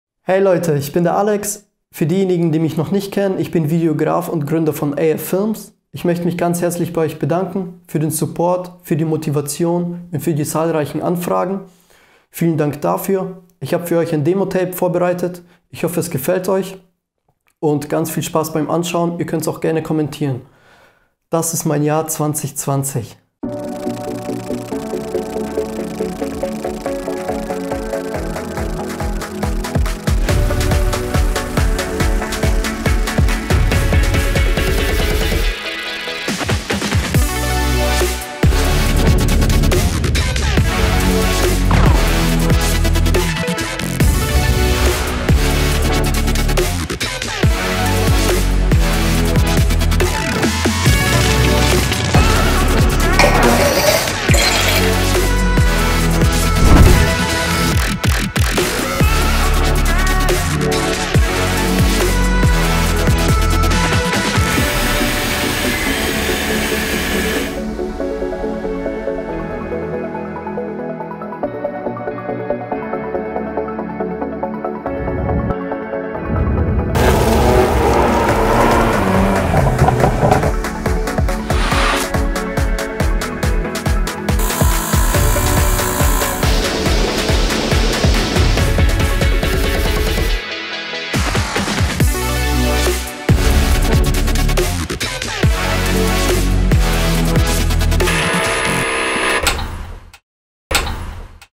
Showreel2020 (Cinematic)
Hallo Leute,
Ich dachte das Jahr 2020, wird sehr spannend werden..
Doch das was kam, war unerwartet.
Trotz allem, hatte ich ein geiles Jahr, voller kreativen Projekten.
Ich blicke mit positiven Gedanken zurück! Und hoffe dass, das neue Jahr 2021! Einfach in jeder Hinsicht noch besser wird!
Vielen Dank an alle, die mich unterstützt und begleitet haben.